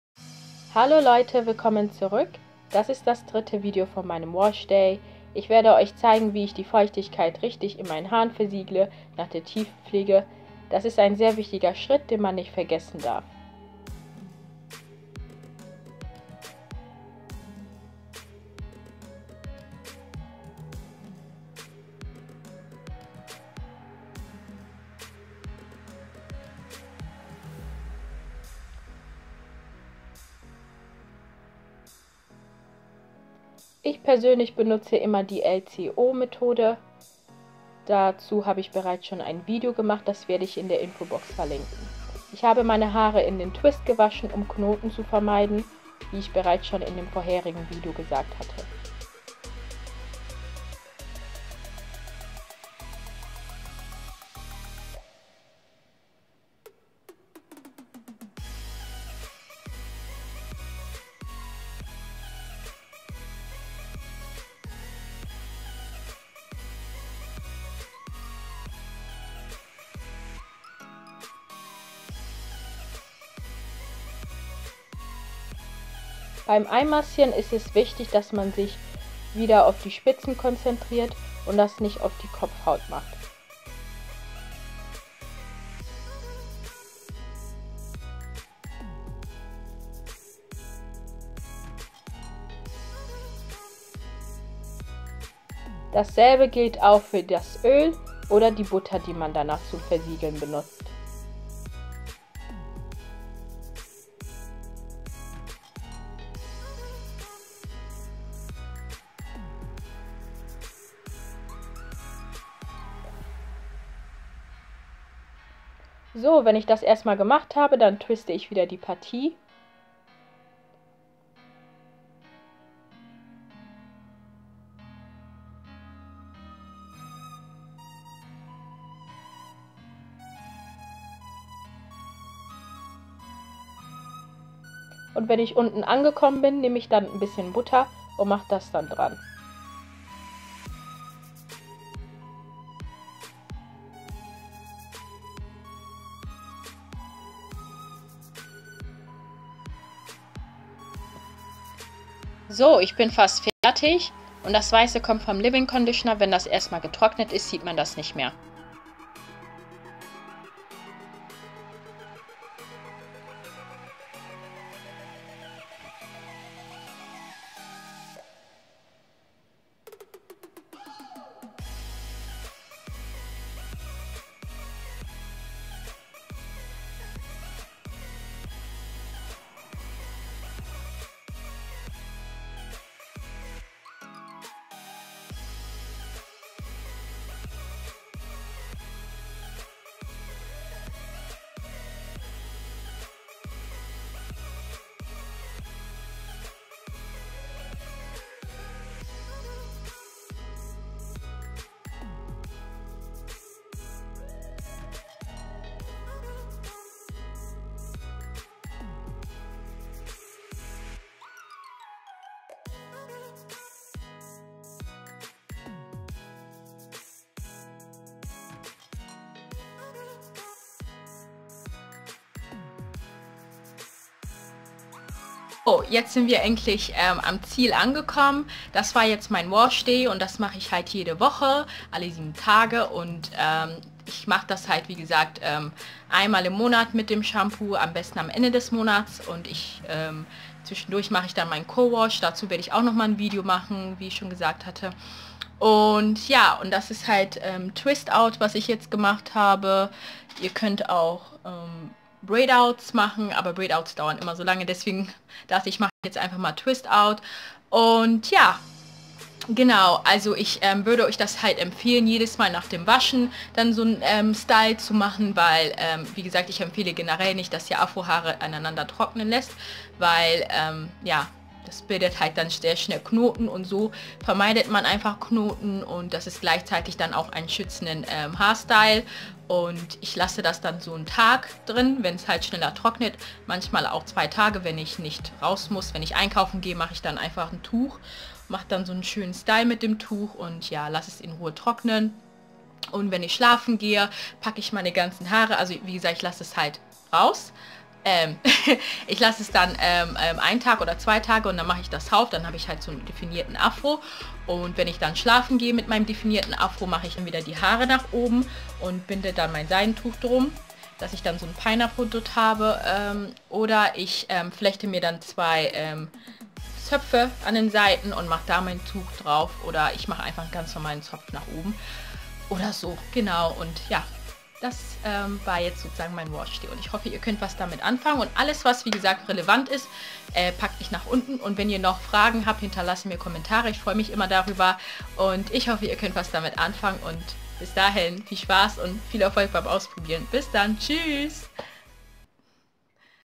Waschtag - Afro Haarpflege Basics Teil 4- Feuchtigkeitsversiegelung // L.C.O Methode 2017
Die richtige Feutigkeitsversiegelung sorgt dafür, dass die Haare bis zur nächsten Haarpflege gut versorgt sind. Sie können in Ruhe alles aufnehmen, was sie brauchen um gesund heranzuwachsen.

Benutzte Sachen:
1. Leave- in Conditioner von Cantu
2. Shea Butter Mix
3. T-Shirt & Handtuch

Viel Erfolg :)